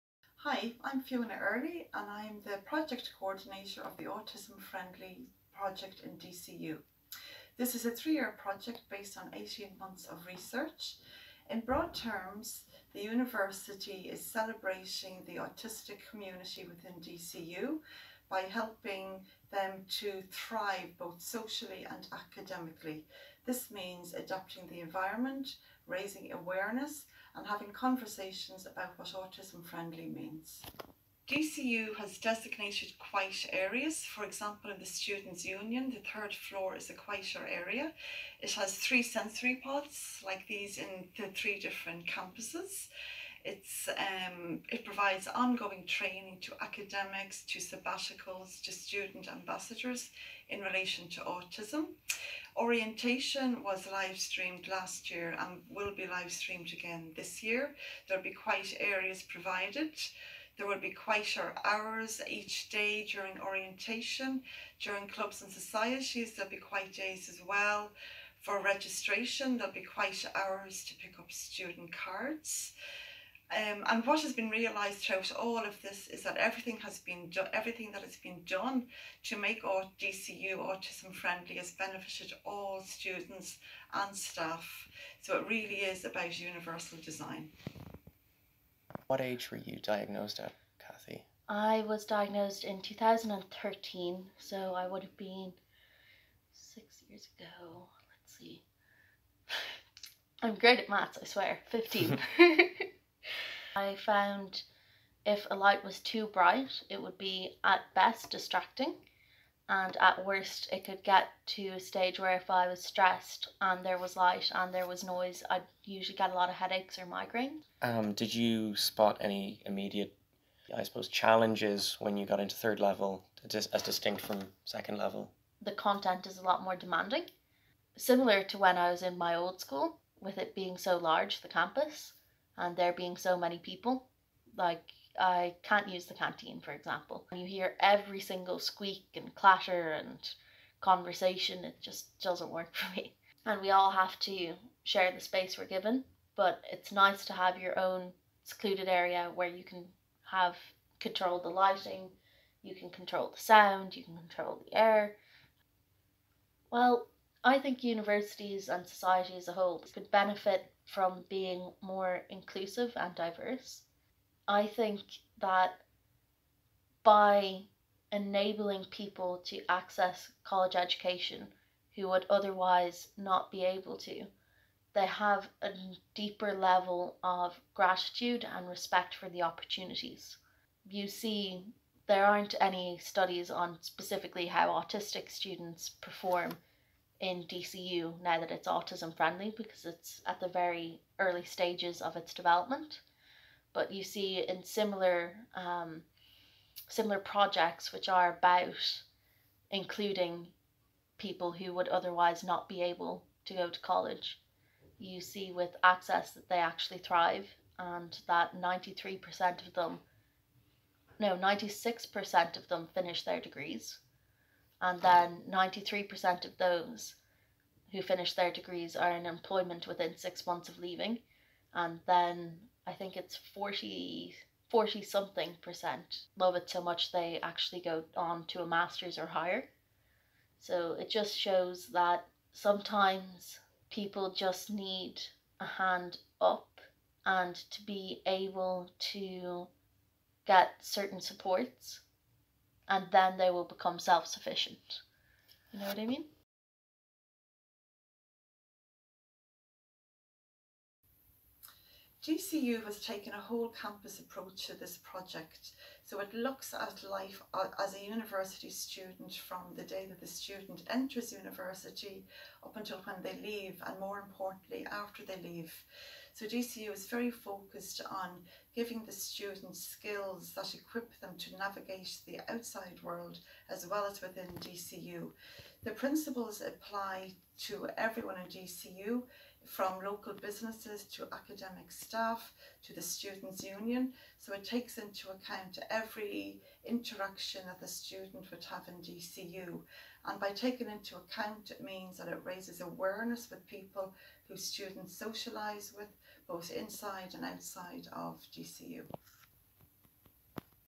DCU Autism Friendly Award
We sat down to talk to Fiona Earley about DCU's new sensory pod which is part of the university's autism friendly measures. DCU is the first third level institution to receive the AsIAm Autism Friendly Award. She talked us through these and then discussed the benefits of these with autistic student Cathy Brennan.